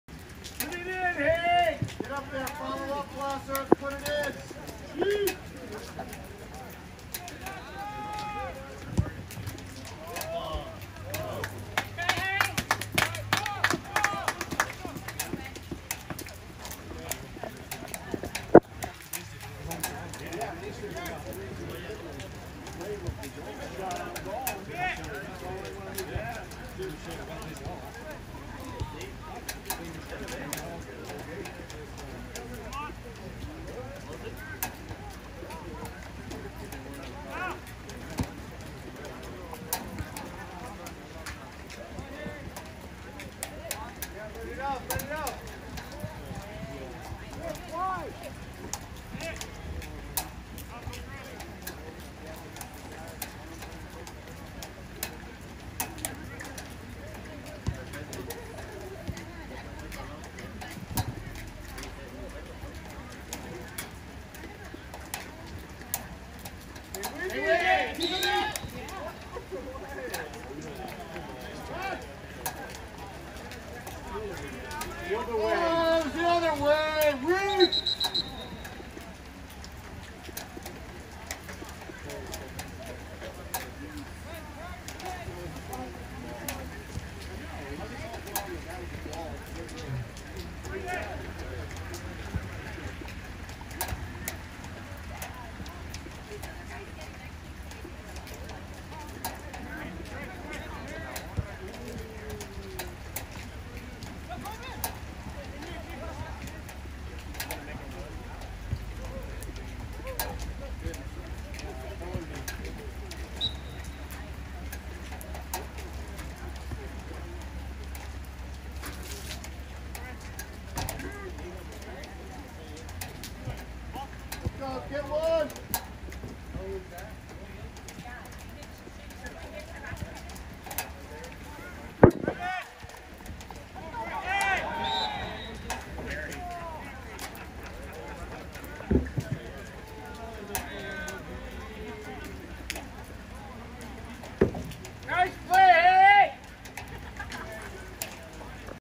#23 TACKLED NECK Foul YELLOW CARD by REFEREE Del Oro Placer Boys Varsity Soccer High
Del Oro High School v Placer High School Soccer RAS Sports Video
Varsity JV Soccer High School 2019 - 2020 Winter Fall Season
Auburn, CA
RAS Sports Video Highlights, Watch Share Enjoy!
RAS Sports
We R Your Biggest Fans!